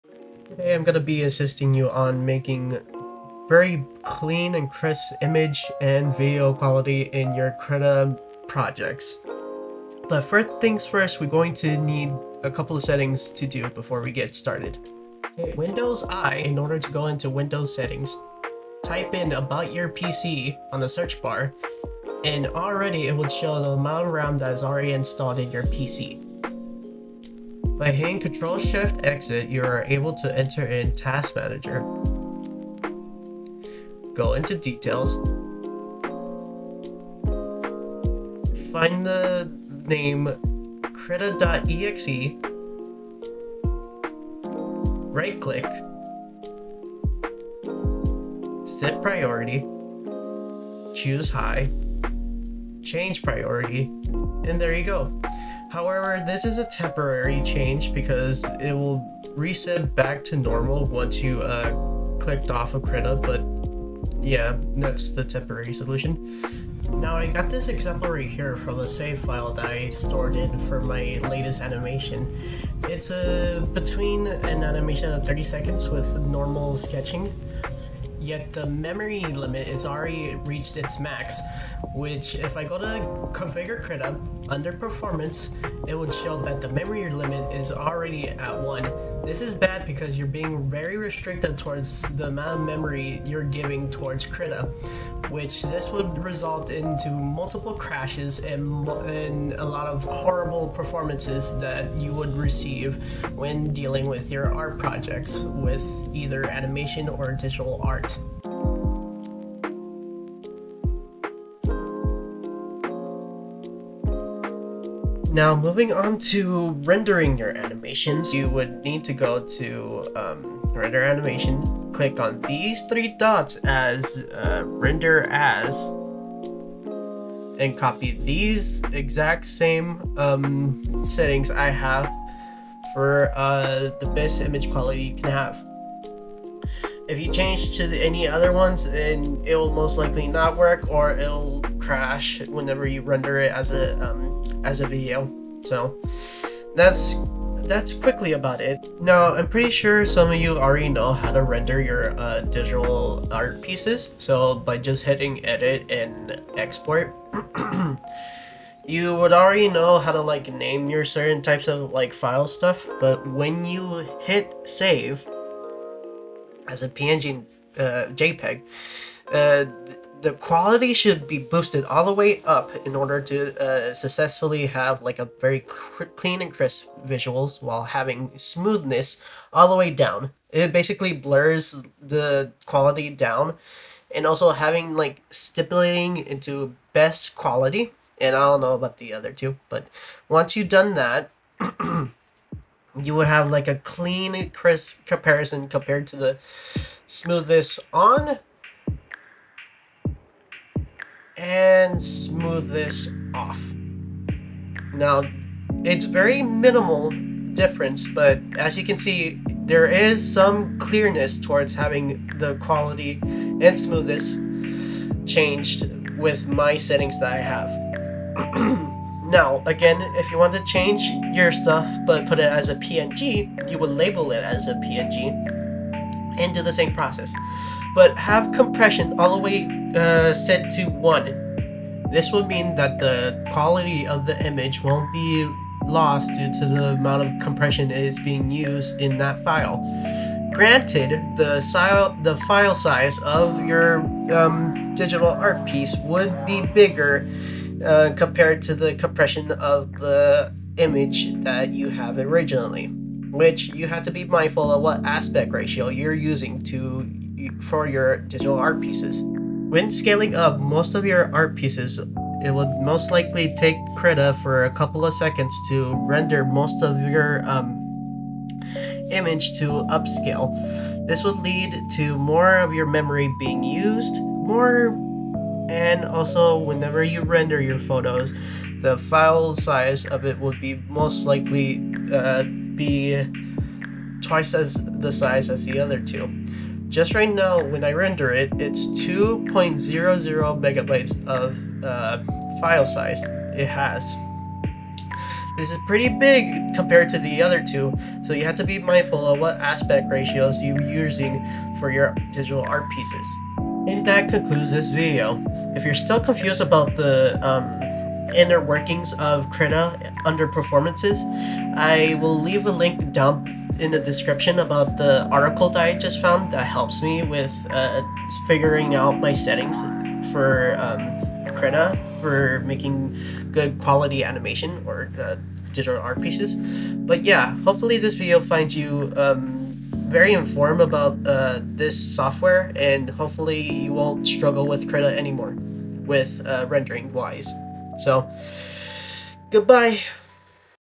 The best krita performance settings
This is not the best editing I ever done nor is it the best voiceover at all. But hey,,,,atleast I tired :,) . Anyway this video is mainly for artist that has performance or rendering problems. So that being said I hope you enjoy the video!! (sorry for the weird times I upload, I have a horrible sleeping schedule)

- Timestamps:

0:00 intro
0:15 troubleshoot your computer
1:05 Performance settings in Krita
1:55 Rendering settings for animations
2:28 export settings for digital art
4:20 examples of exporting images example
5:03 outro